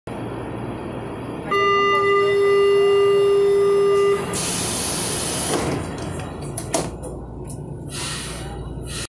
Fechamento das portas das Frotas I/L
Filmado no L28 (carro L284)
Data: 19/10/2019

Frota L (L28) fechando as portas na estação São Joaquim sentido Tucuruvi